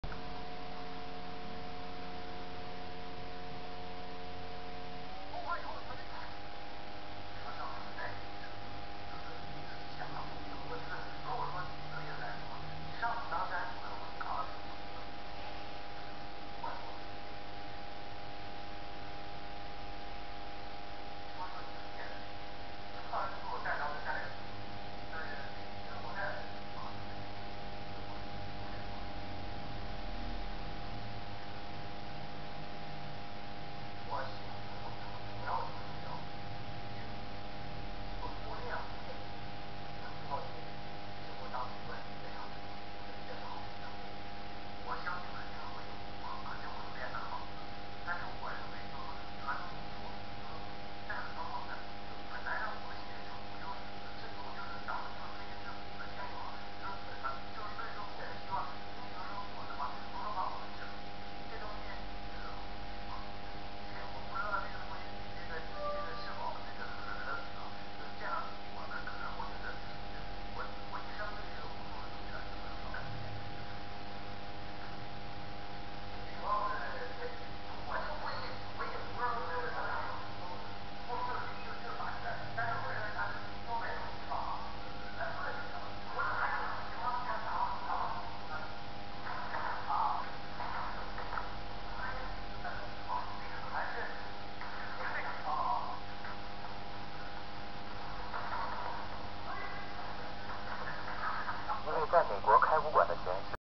very rare fight Shaolin Shi De Jian vs Heilongjiang Boxing champion
it's about a full speed fight
Shaolin master Shi De Jian vs Boxing champion
the champion didn't look up to Shaolin Kung fu at first.
and they had an arguement at the wrap of the conversation.
this link for good quality and where is this video from.